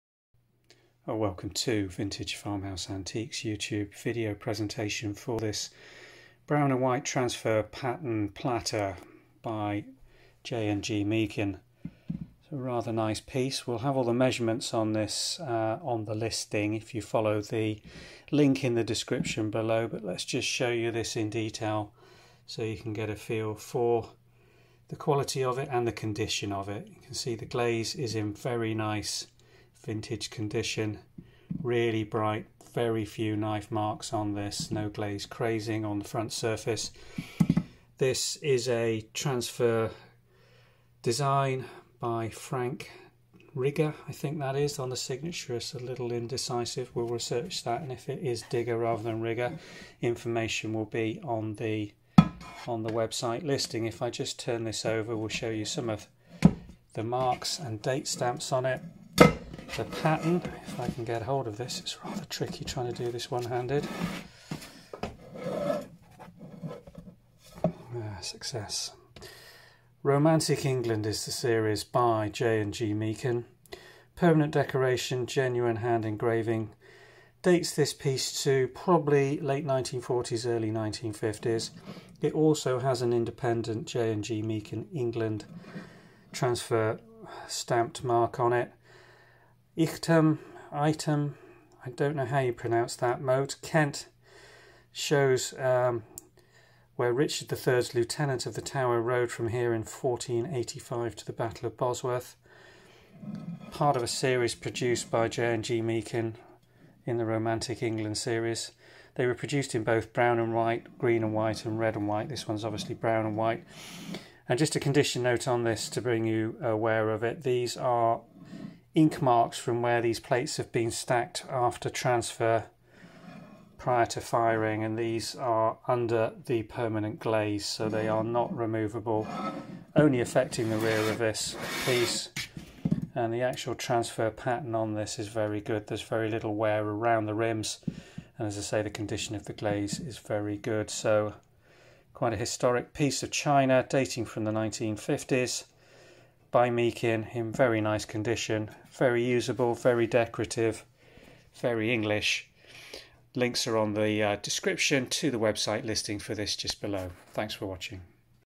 Meakin Romantic England China Platter
J & G  Meakin vintage oval platter from the Romatic England series ironstone china
Classic English vintage china dating from 1950's
Medium oval serving platter in brown and white transfer ware
12.25-inch by 9-3/8th-inch oval shallow serving platter depicting Ightham Mote
From the Romatic England series of china
1950's J G Meakin factory stamps
In very good condition but please note the areas of brawn transfer markings from firing on the rear of the platter, shown in better detail in the video.